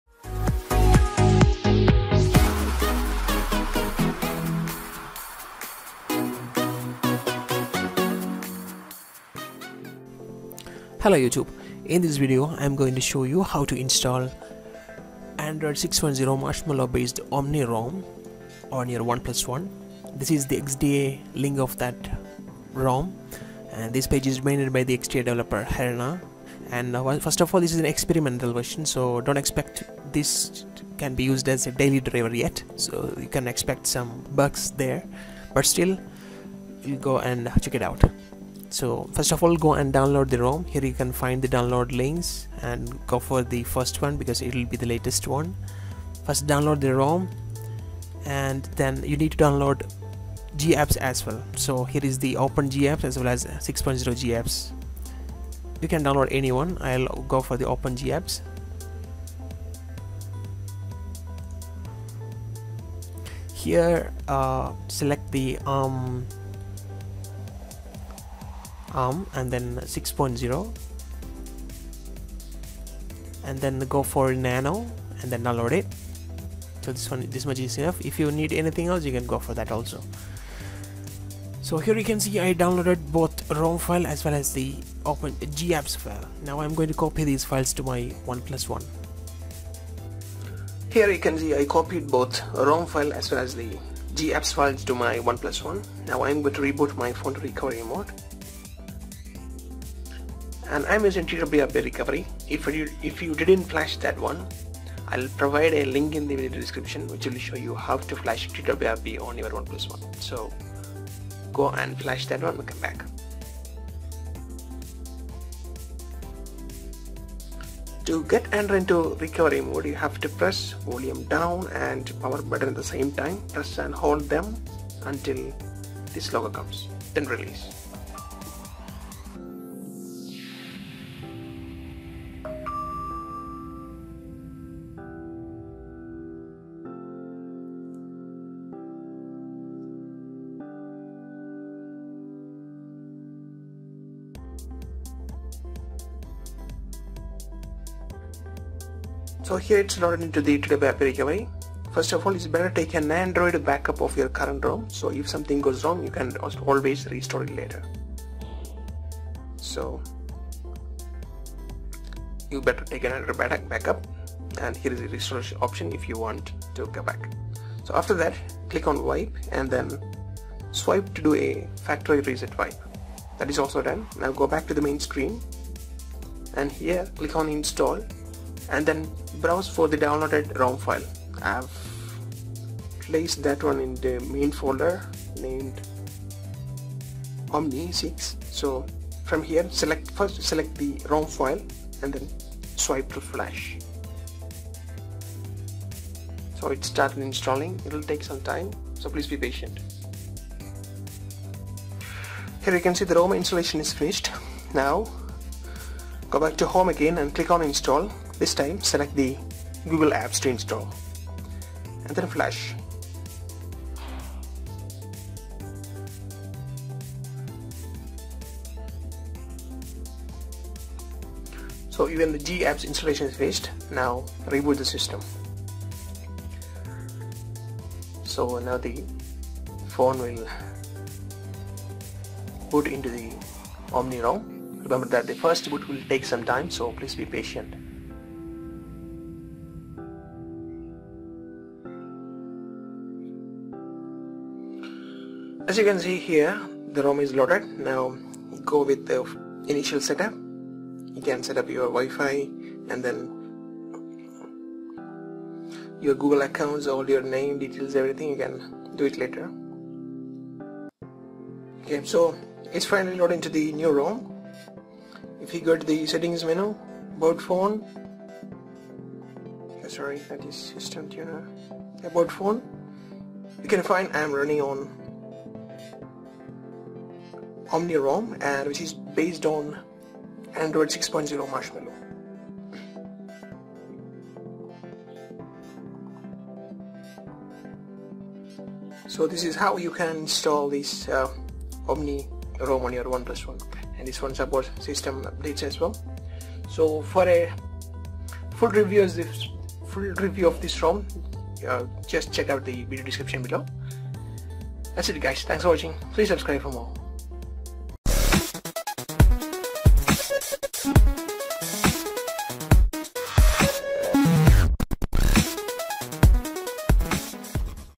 How to Install OmniROM Marshmallow 6.0 ROM on OnePlus One
This video will show you How to Install OmniROM Marshmallow 6.0 ROM on OnePlus One.

Get OnePlus One without Invite